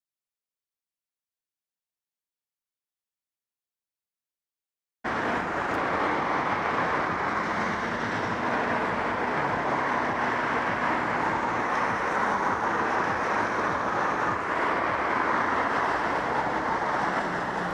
FE03KZC where do you mount your gps
This driver likes his just to the right of the center of his windscreen, perfect for blocking a substantial area of the road ahead.

I've been seeing a lot of people mounting their gps, phone, whatever in the middle, or close to the middle, of their windscreen. They must be really sure they won't ever need to look out of that bit of the windscreen.

Thursday 29th October 2015 approx 17:10